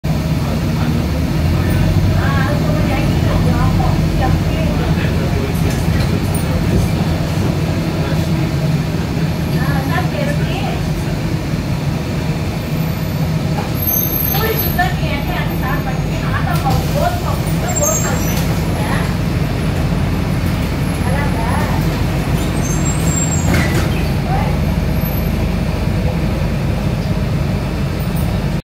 rapidPenang Bus On The Move With Front Door Open
Rapid Penang PJJ4735 on Route 303.

Filmed on a Friday, ‎23 December ‎2022 in Malaysia.

Featuring showcase overview a Scania K series bus with Gemilang Coachwork Sdn Bhd bodywork.

Rapid Bus Sdn Bhd,  Prasarana Malaysia Berhad ﾠﾠ
ﾠﾠ